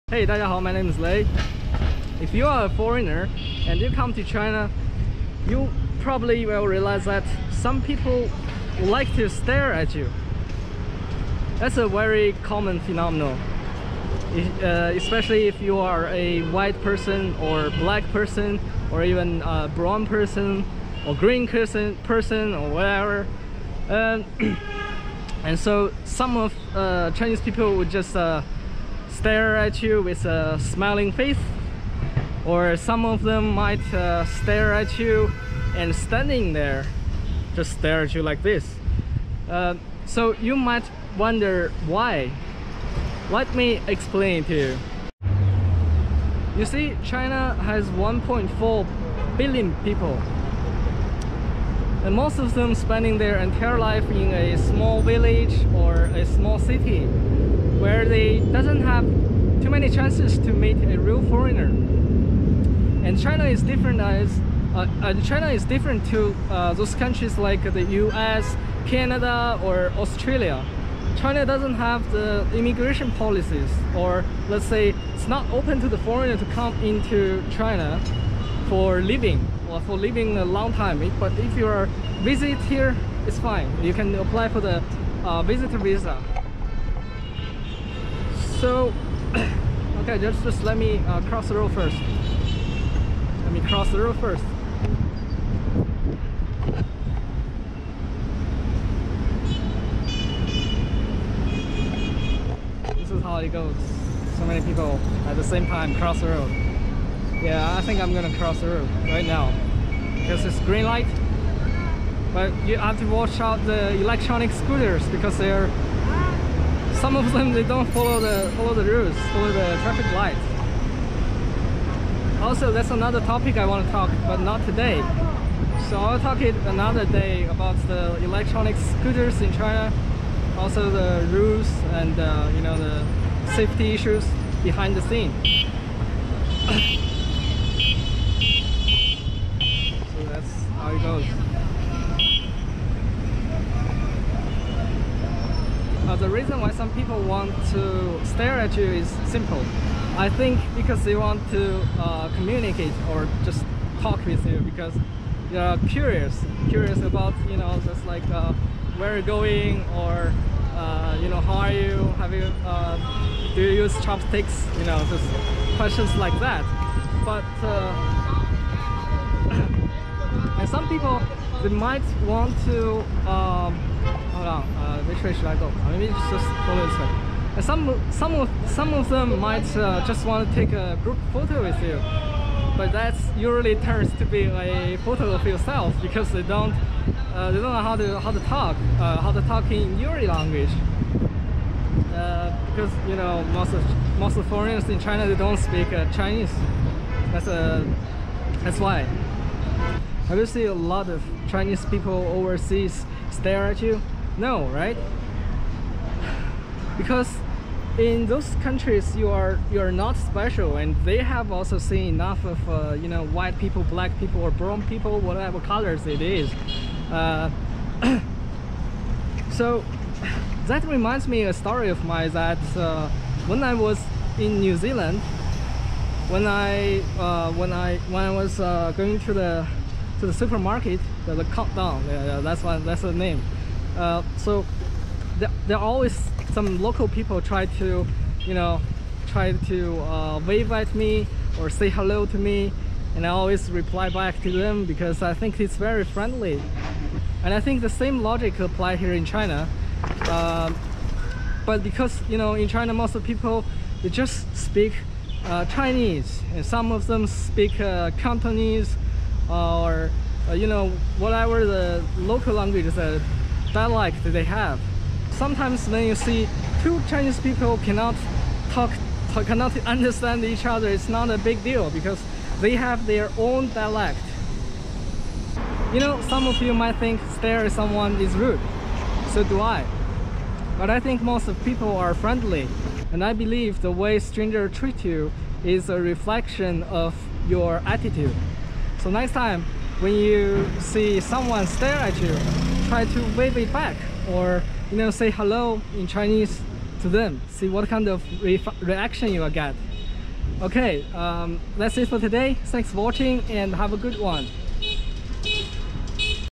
Why Chinese People Stare at You - Shenzhen Vlog
Have you ever been stared at? Why people in China do it a lot to other people? Here is my answer...